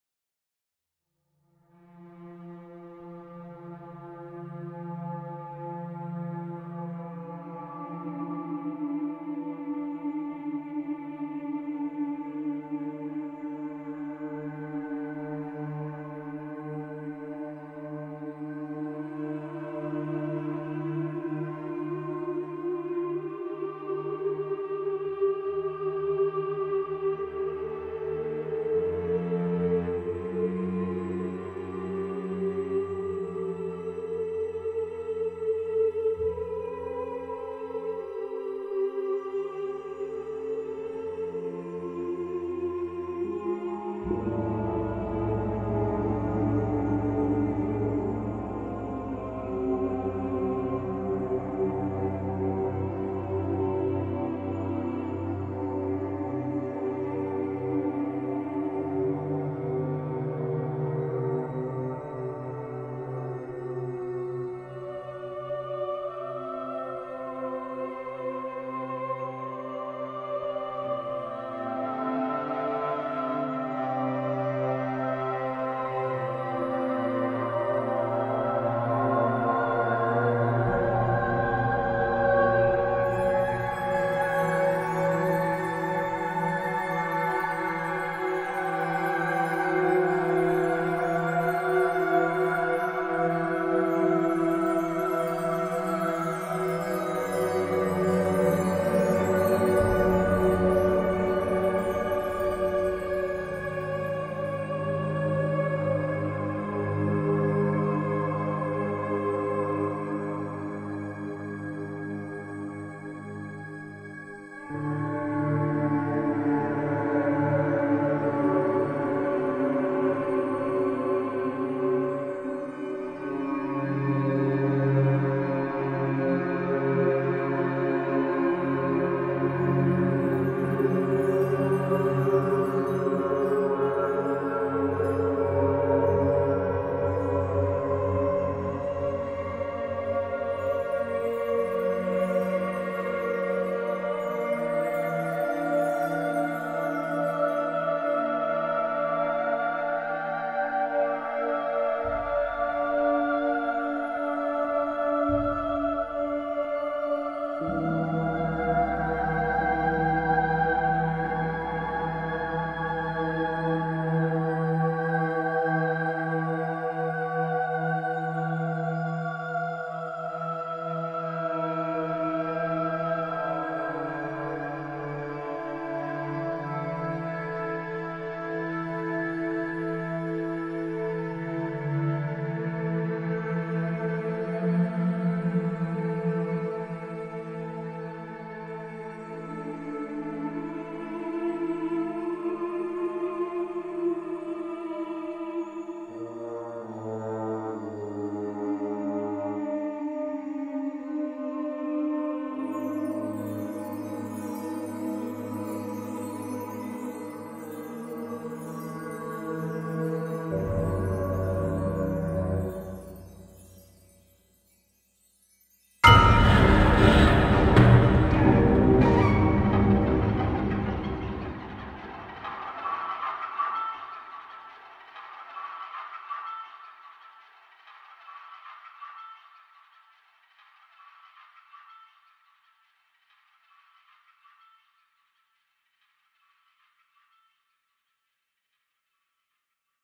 Haunted
A short mysterious movie.
describing the spooky and gloomy atmosphere of a haunted castle ruin.
Music and video by Co Oudhoff.